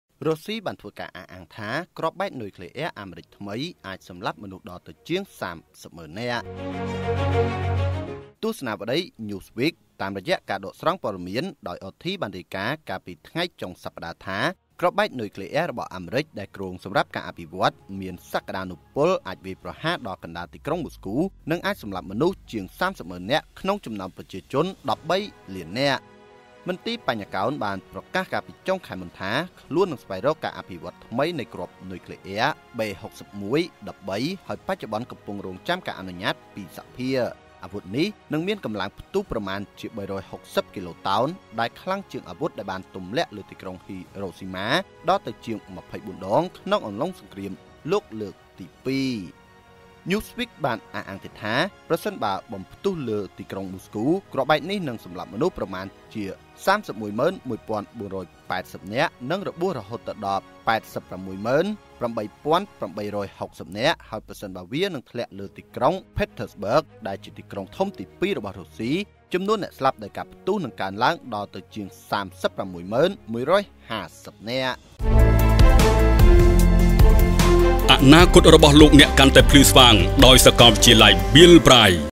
រុស្សី៖ គ្រាប់បែកនុយក្លេអ៊ែរអាមេរិកថ្មី អាចសម្លាប់មនុស្សដល់ទៅជាង៣០ម៉ឺននាក់
VIDEO៖ រុស្សី៖ គ្រាប់បែកនុយក្លេអ៊ែរអាមេរិកថ្មី អាចសម្លាប់មនុស្សដល់ទៅជាង៣០ម៉ឺននាក់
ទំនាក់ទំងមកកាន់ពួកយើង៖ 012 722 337